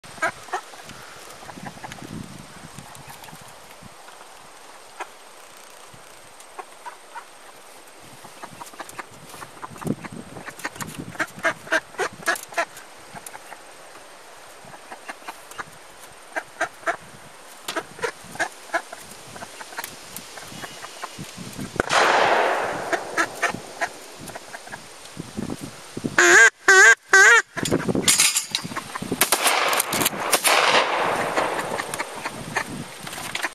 Live Flyer / Live Fire Senior Hunt Test, Sunday 5 Sept. 2010, Upstate NY
Think of it as a Funnelator for Ducks.  No dogs or humans were harmed in the making of this video, alas, the ducks didn't fare as well, though one did manage to escape, we think unharmed.